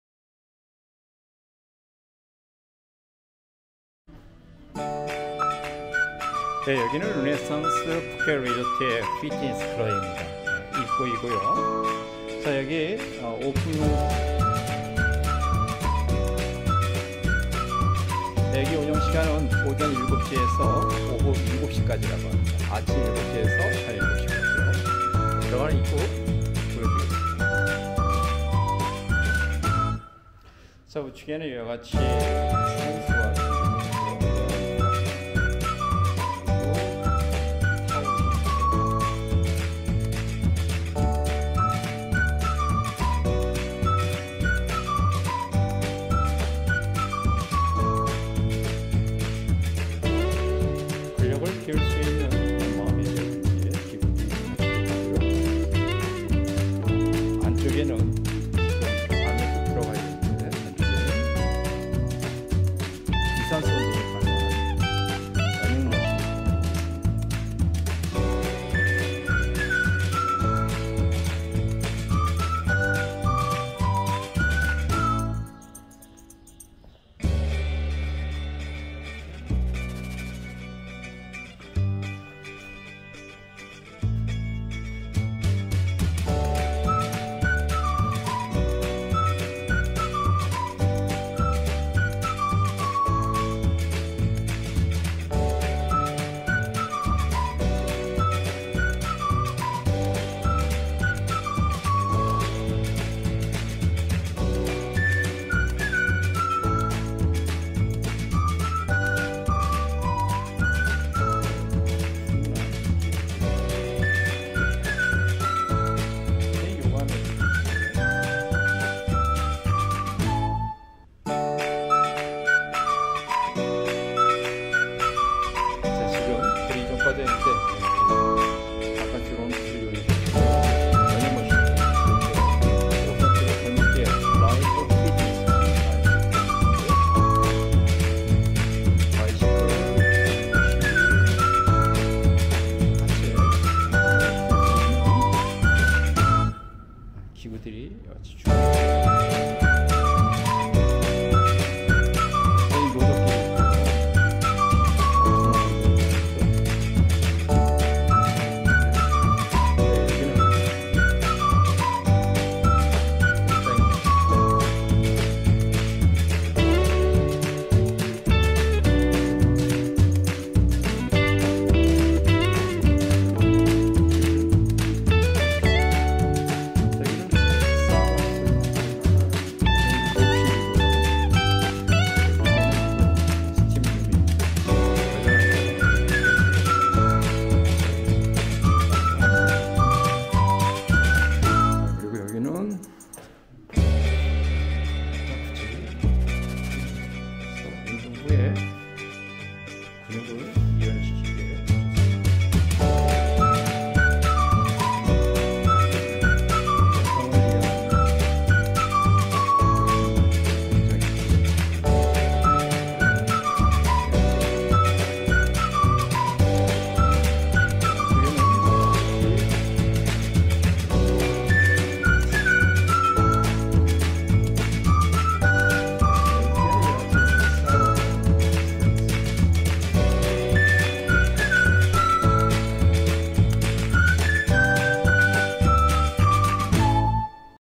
푸켓 르네상스 마이카오비치 리조트 피트니스클럽 태초클럽 Phuket Renaissance Resort Fitness Club
르네상스 푸켓리조트는 푸켓공항 북쪽 마이카오비치에 위치한 해변 리조트이자 풀빌라 리조트입니다. 매리어트 계열의 5성급 리조트로 멋진 자연,해변접근성, 좋은 시설, 깔끔한 룸, 좋은 서비스로 인기가 있습니다.

피트니스클럽은 5성급 리조트로 무난합니다. 라이프 피트니스 제품의 고급기기들로 운동하기에 좋습니다.

태초클럽 태국전문여행사로 호텔,투어,쇼,픽업등의 여행업 관련 서비스를 제공합니다.